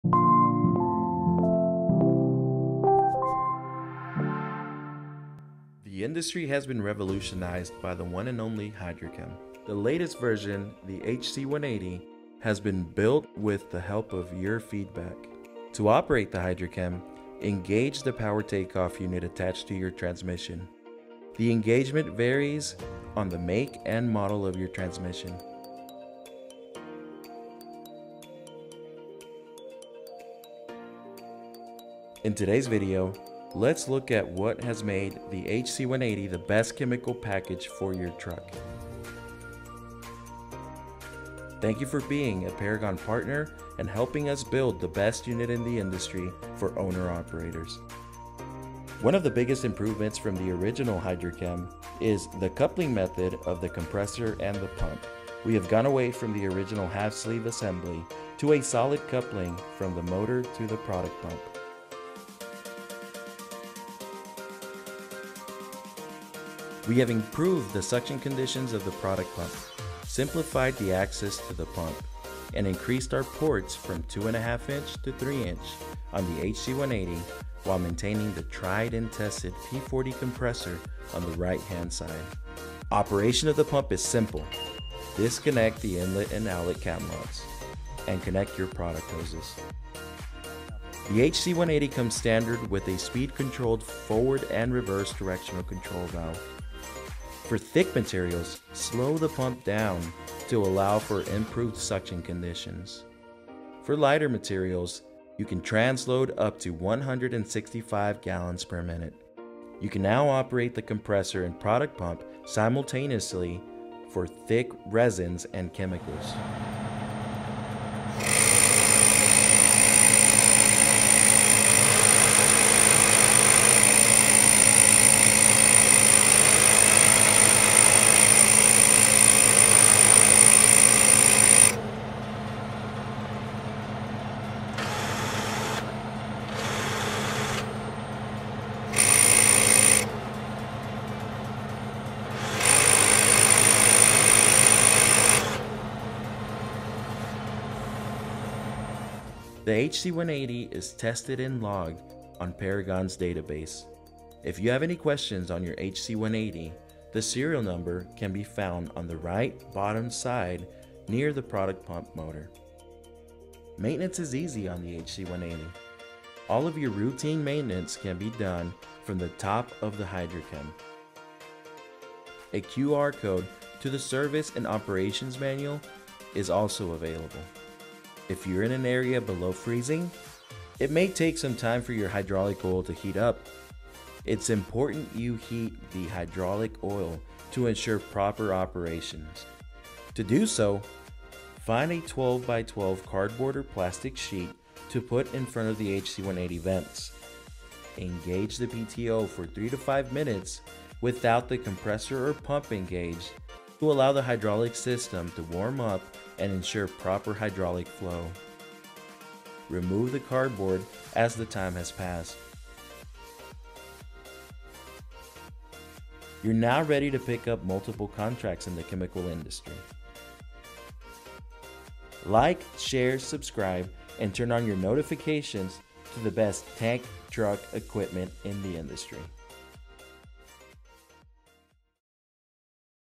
HydraCHEM® HC180 Features and Operation
Self Contained unit includes:
- Hydraulic cooler
- Hydraulic Return Filtration
- Chemical Product Pump
- Air Compressor
- Hydraulic Reservoir
The compact design shaves 100lbs+ off of a typical hydraulic system weight.
Easy installation cuts down on installation time by roughly 4 hrs.
Installation Requirements:
- Minimum of 25" of frame rail space.
- Only four Mounting holes required.
- Two Hydraulic Hoses.
- Simple operation design places everything within the operator's reach.
Performance Information:
- Product Pump Delivery: Up to 165 GPM @ 100 PSI
- Compressor Delivery: Up to 35 CFM @ 25 PSI
- Gear Pump or Lobe Pump options available.